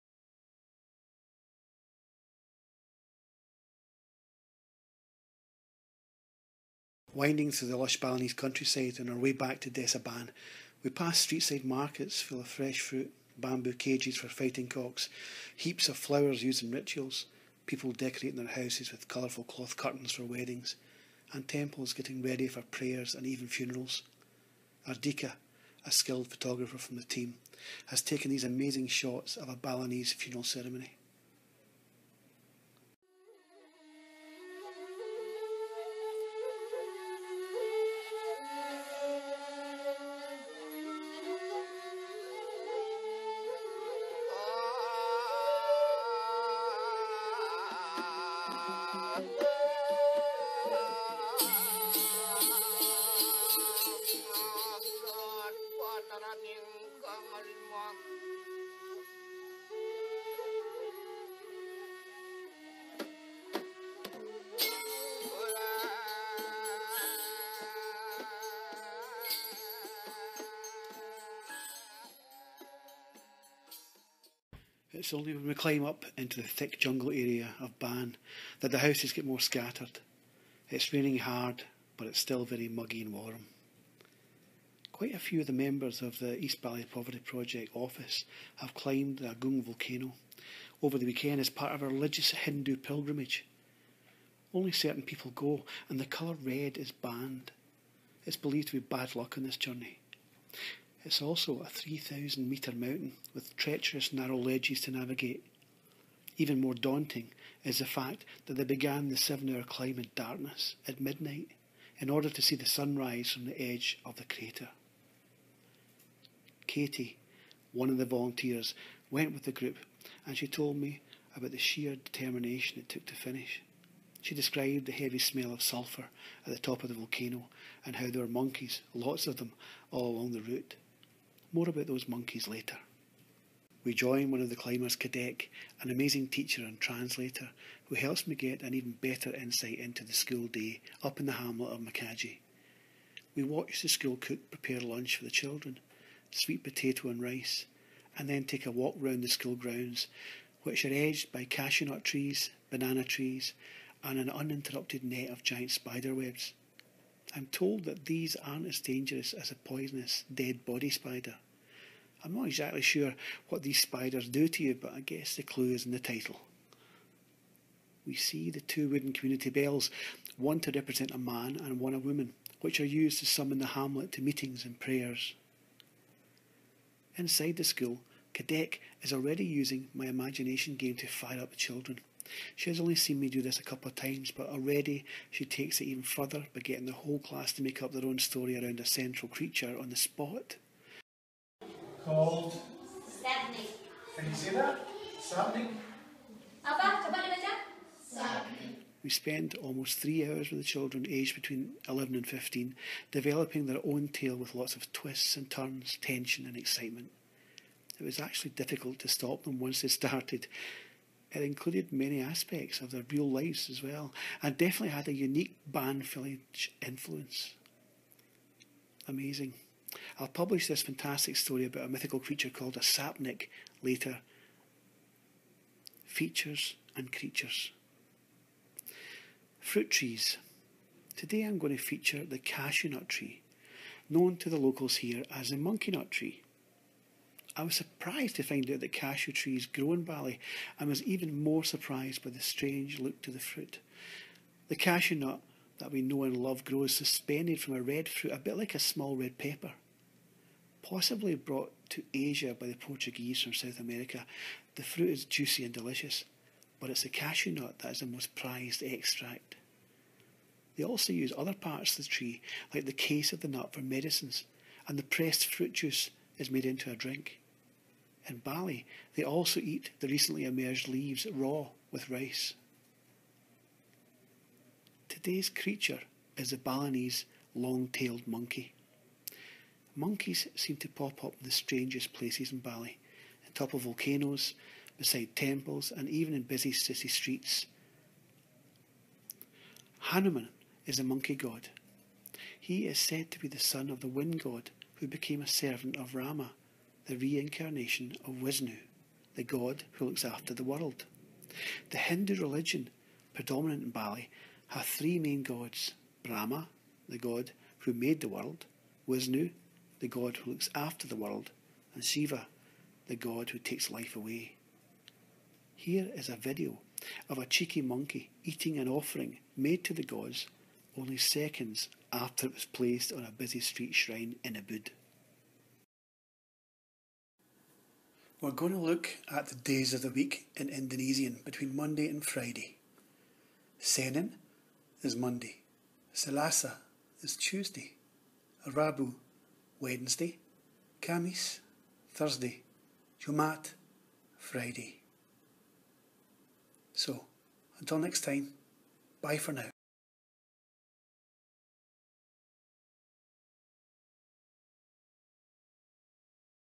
Paul Murdoch's Window on Another World Bali 2015 Part 6
Winding through the lush Balinese countryside on our way back to Desa Ban, we pass street-side markets full of fresh fruit, bamboo cages for fighting cocks, heaps of flowers used in rituals, people decorating their houses with colorful cloth curtains for weddings, and temples getting ready for prayers and even funerals. Ardika, an skilled photographer from the team, has taken these amazing shots of a Balinese funeral ceremony. It's only when we climb up into the thick jungle area of Ban that the houses get more scattered. It's raining hard but it's still very muggy and warm. Quite a few members of the EBPP office have climbed the Agung volcano over the weekend as part of a religious Hindu pilgrimage. Only certain people go and colour red is forbidden as it is believed to be bad luck on this journey. It's also a three thousand meter mountain, with treacherous narrow ledges to navigate. Even more daunting is the fact that they began the seven hour climb in darkness, at midnight, in order to see the sun rise from the edge of the crater. Katie, one of the volunteers, went with group and told me about the shear determination it took to finish. She described the heavy smell of sulphur at the top of the volcano and how there were monkeys, lots of them, all along the route. More about those monkeys later...

We join one of the climbers, Kadek, an amazing teacher and translator who helps me get an even better insight into the school day up in the hamlet of Manikaji Pucak. We watch the school cook prepare lunch for the children, sweet potato and rice, and take a walk round the school grounds, which are edged by cashew nut trees, banana trees and an uninterrupted net of giant spider webs. I'm told that these aren't as dangerous as the poisonous 'dead body spider'. I'm not exactly sure what these spiders do to you but I guess the clue is in the title. We see the two wooden community bells, one to represent a man and one a woman, which are used to summon the hamlet to meetings and prayers.
Inside the school, Kadek is already using my imagination game to fire up the children. She has only seen me do this a couple of times but already she takes it even further by getting the whole class to make up their own story around our central creature, on the spot. We spend almost three hours with children, aged 11 to 15, developing their own tale with lots of twists and turns, tension and excitement. It was actually difficult to stop them once they had started. It included many aspects if their real lives and definitely had a unique Ban Village influence. Amazing. I'll publish this fantastic story about a mythical creature called a Sapnik in a later update.

Features and Creatures

Fruit trees...

Today I'm going to feature the Cashew Nut Tree, known to the locals here as the 'monkey nut tree'.

I was surprised to find out that the cashew tree grows in Bali. I was even more surprised by the  strange look of the fruit. The cashew nut that we know and love grows, suspended from a red fruit a bit like a small red pepper. Possibly brought to Asia by the Portuguese from South America, the fruit is juicy and delicious, but it is the cashew nut that is the most prized extract from the tree. They also use other parts of the tree, like the case of the nut for medicines and the pressed fruit juice is made into a drink. In Bali they also eat the recently emerged leaves raw with rice.

Today's creature is the Balinese Long-tailed Monkey.

Monkeys seem to pop up in the strangest places in Bali. On top of volcanoes, beside the temples and even in busy city streets. Hanuman is the monkey god. He is said to be the son of the wind god, who became a servant of Rama, a reincarnation of Wisnu, the God who looks after the world. The Hindu religion, predominant in Bali, have three main gods - Brahma (the god who made the world), Wisnu (the god who looks after the world) and Siva (the god who takes life away). Here is a video of a cheeky monkey eating an offering made to the gods only seconds before it was placed on busy street shrine in Ubud.